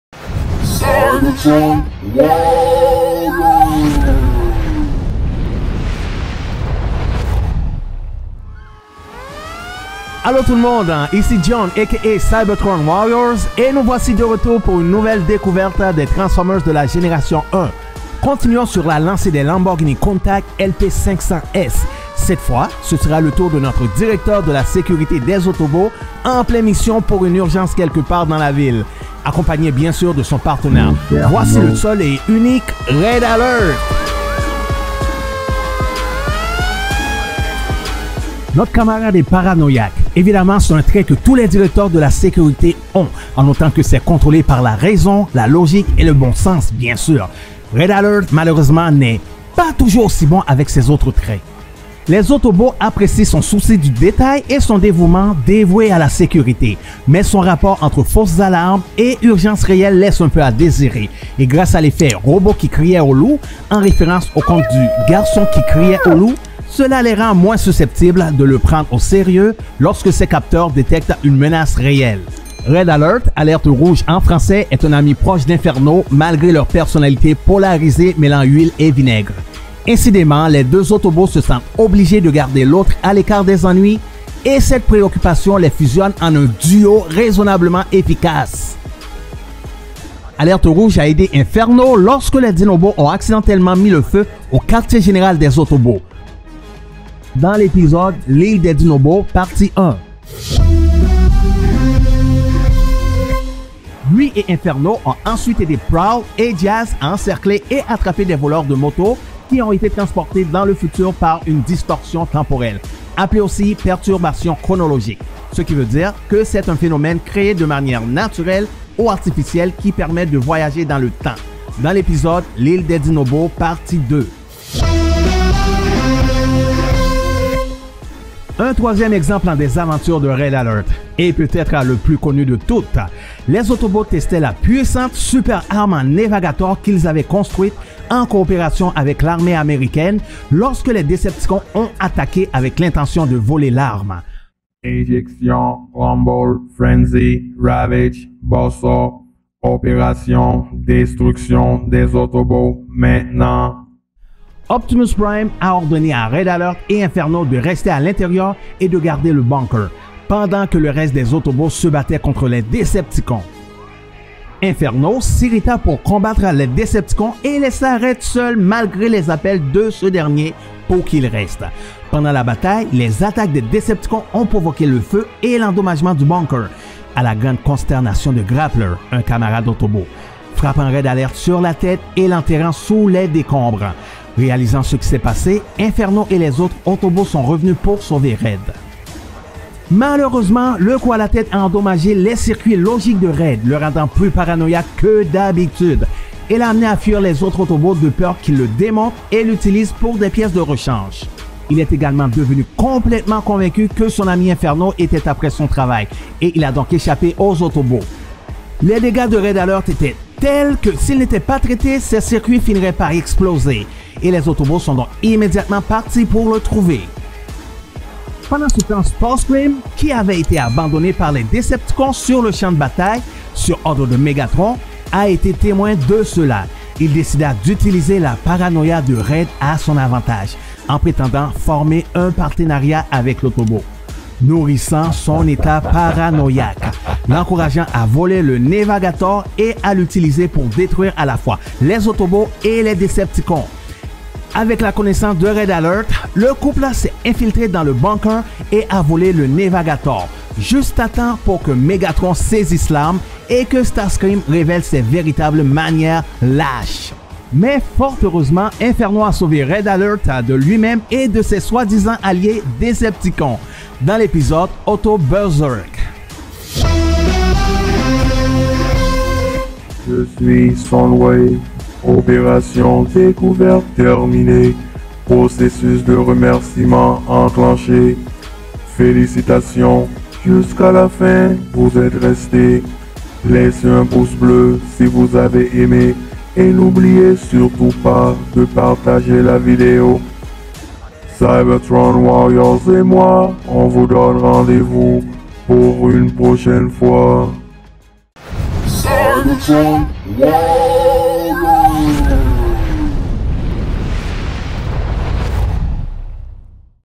Découverte Masterpiece MP-14 Red Alert (Version courte, sans vidéos)
Bonjour à tous, nous voici rendu à la 8e épisode de la série découverte des Transformers de la génération G1. Cette fois, c'est au tour de Red Alert de se faire connaître .